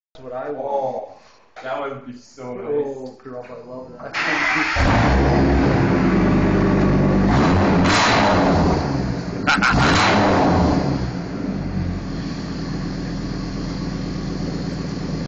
Every Fire hall needs one
Went on a tour of some fire halls in the Crows nest pass and had to post this truck that one of the fire halls had. Awesome sounding with a foam unit on the back. Sweet truck.